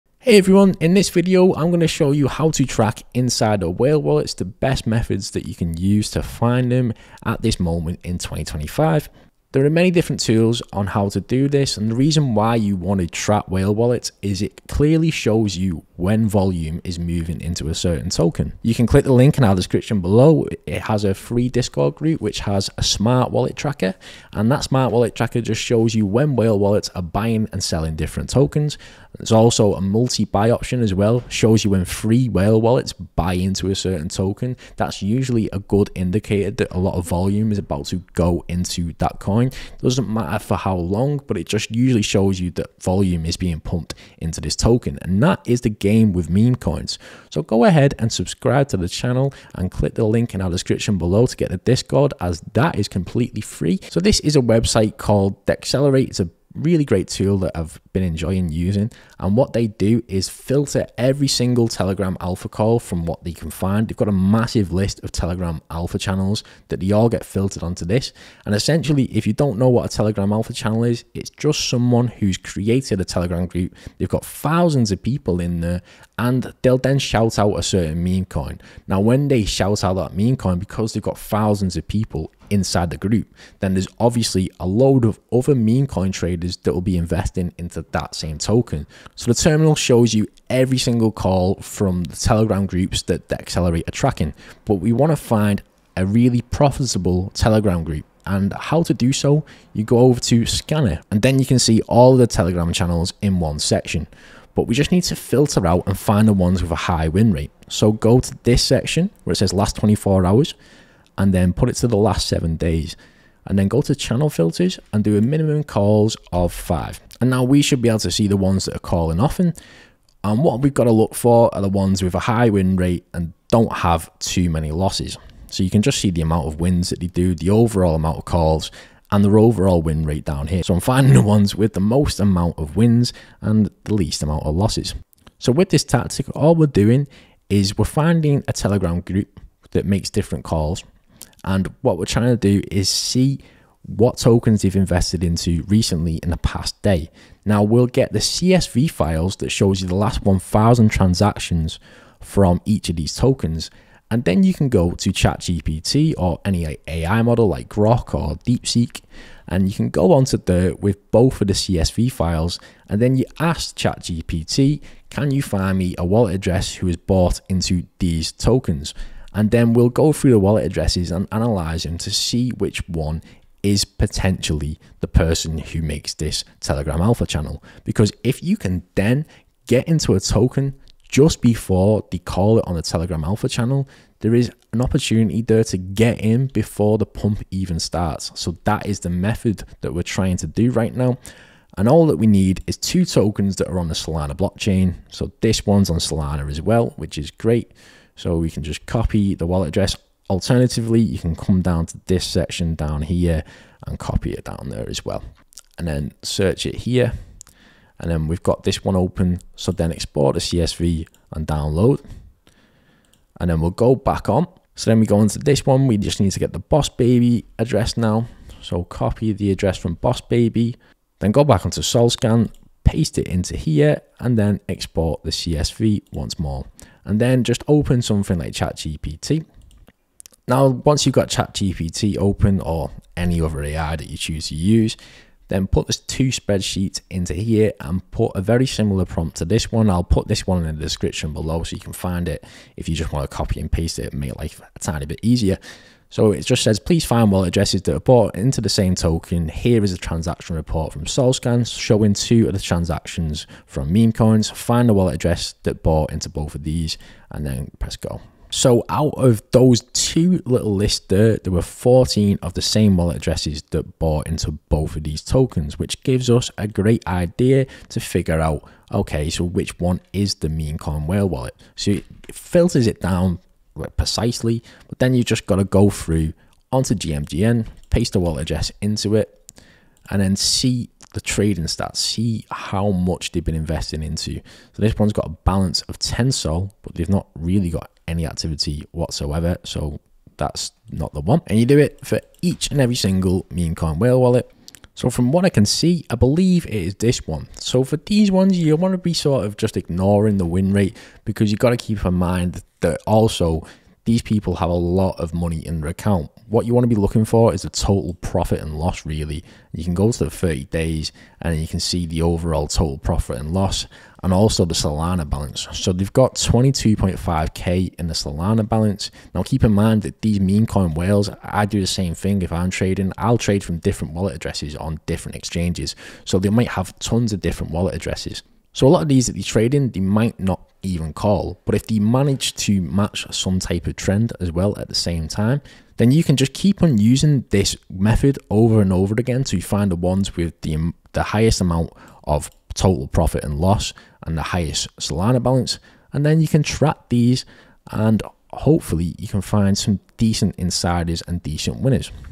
How To Find The Wallet Addresses Of Telegram Alpha Channels (FREE TELEGRAM CALL SCRAPER)
Discover a powerful method to uncover wallet addresses from Telegram alpha channels using free tools! In this video, I’ll walk you through how I use Dexcelerate to track down Telegram alpha calls and analyze their past performance. Then, I’ll show you how to leverage ChatGPT to identify the wallet addresses buying into the same tokens. Whether you're scouting crypto opportunities or just curious about the process, this step-by-step guide is perfect for beginners and seasoned traders alike.

🔑 What You’ll Learn:
- How to use Dexcelerate to find Telegram alpha calls
- Analyzing past calls for better insights
- Using ChatGPT to pinpoint wallet addresses tied to specific tokens
- Tips for scraping Telegram data for free

Drop a comment if you’ve tried this method or have questions—I’d love to hear from you!